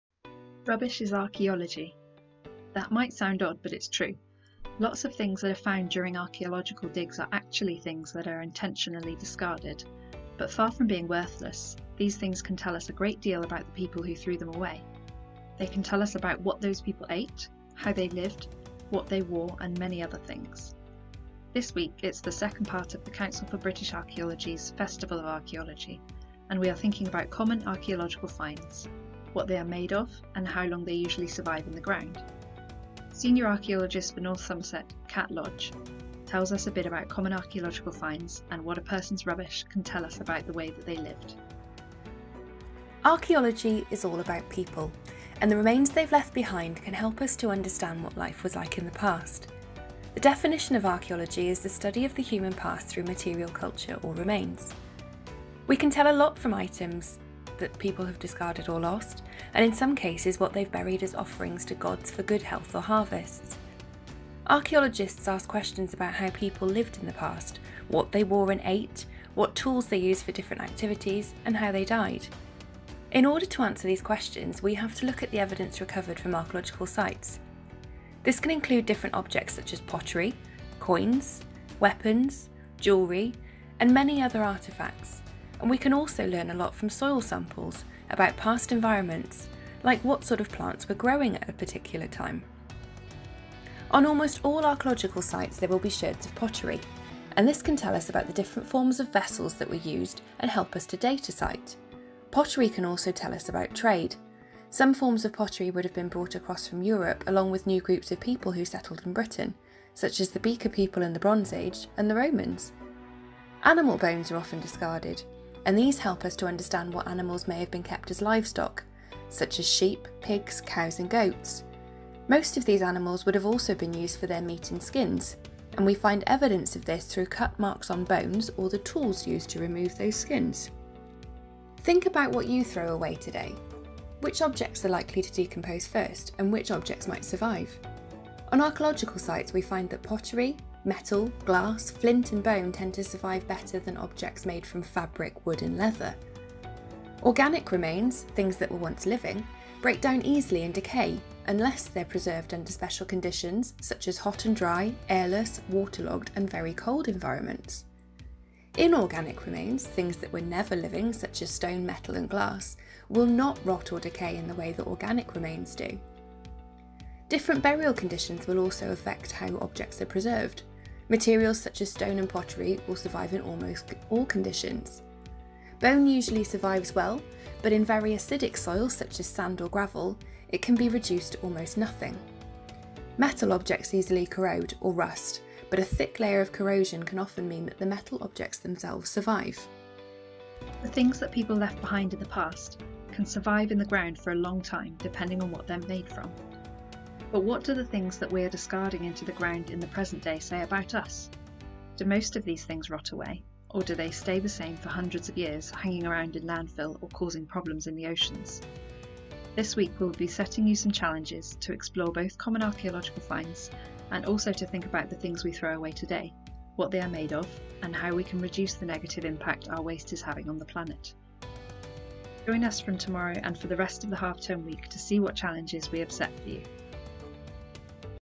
Rubbish Archaeology - A Set of Challenges for Mini Archaeologists
Rubbish is Archaeology! That might sound odd, but it’s true. Lots of things that are found during archaeological digs are actually things that are intentionally discarded. But far from being worthless, these things can tell us a great deal about the people who threw them away. They can tell us about what those people ate, how they lived, what they wore and many other things.

This week, it’s the second part of the Council for British Archaeology’s Festival of Archaeology and we are thinking about common archaeological finds; what they are made of and how long they usually survive in the ground.

Senior Archaeologist for North Somerset, Cat Lodge, tells us a bit about common archaeological finds and what a person’s rubbish can tell us about the way that they lived.

For the Festival of Archaeology, we will be setting you some challenges to explore both common archaeological finds and also to think about the things we throw away today; what they are made of and how we can reduce the negative impact our waste is having on the planet. These challenges will be posted on our website blog and social media, so keep an eye out over the coming week!

Image Credits: Avon Archaeology Ltd, Wessex Archaeology